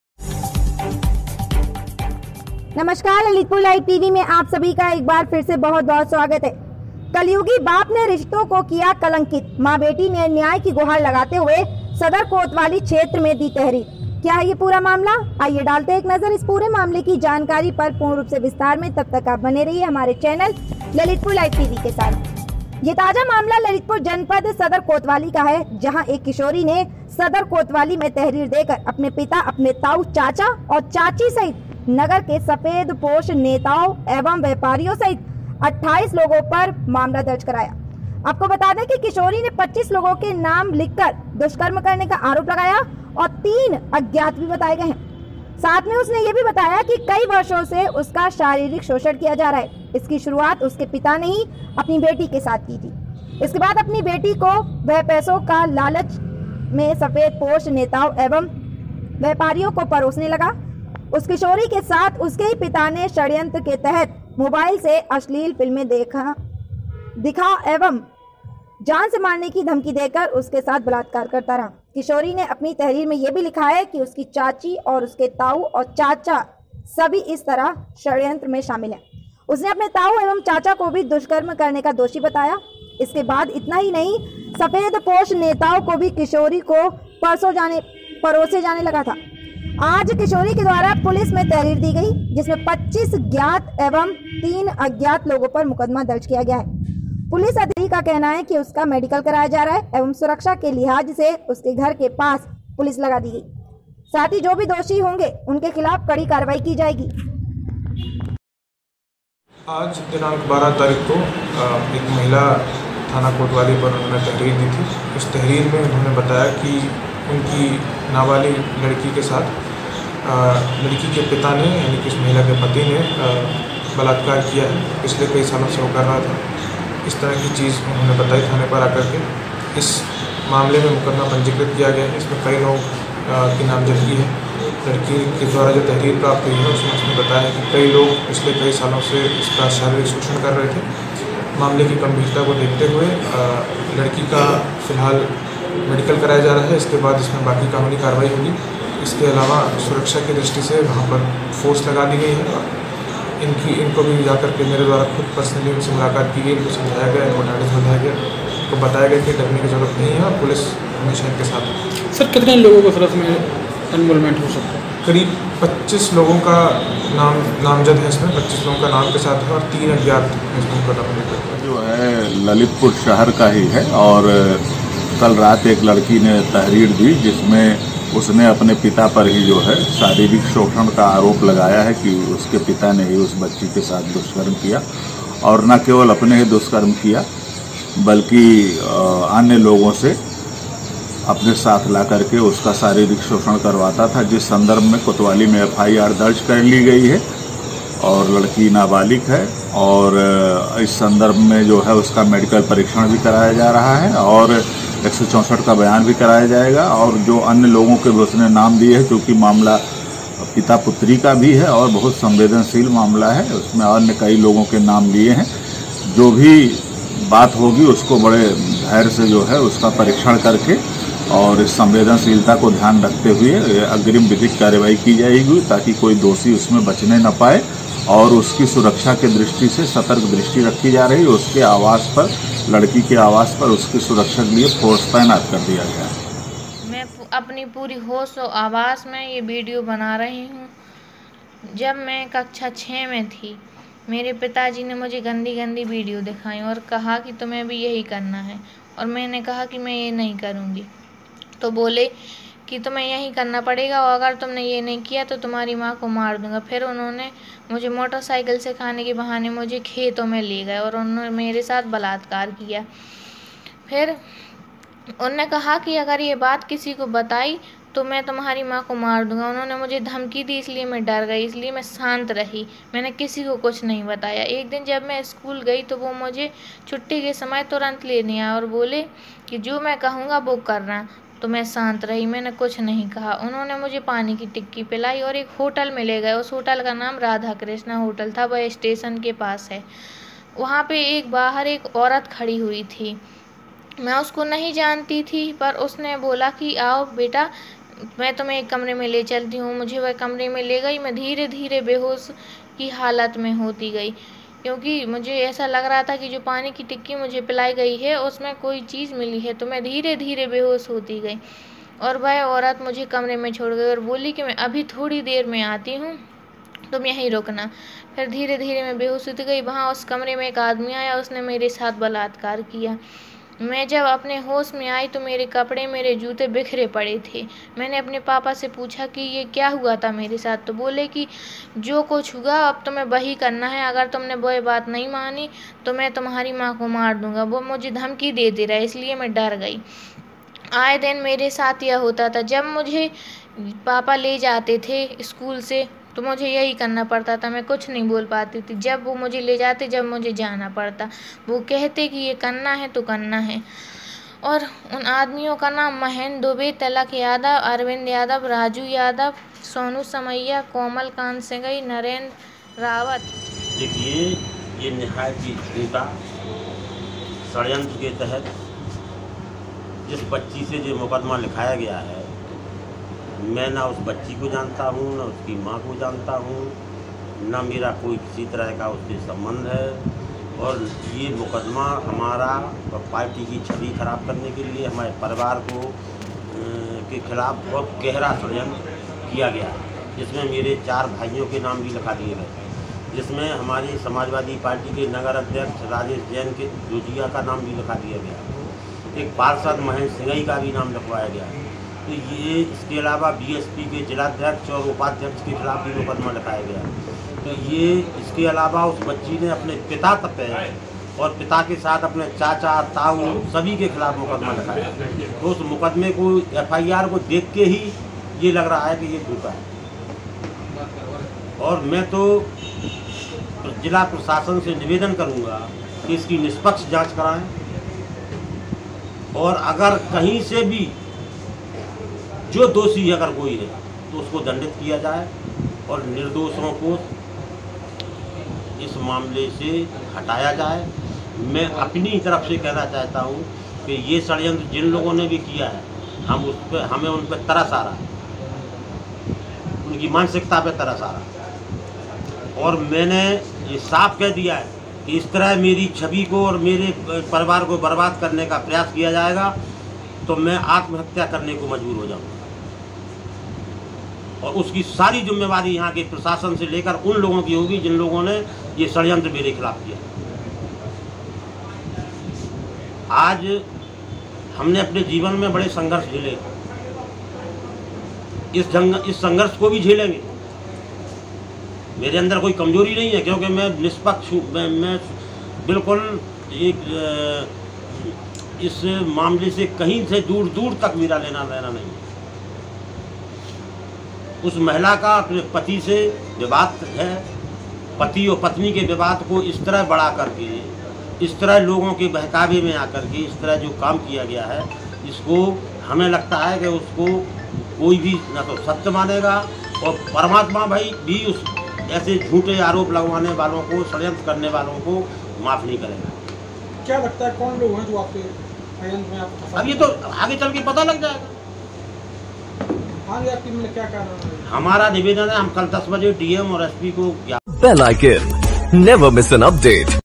ललितपुर में रिश्तो को शर्मसार कर देने वाली घटना नाबालिक पुत्री ने ही पिता को बताया दरिंदा इस पूरे
मामले को लेकर पुत्री का क्या है कहना सुनिए रिपोर्ट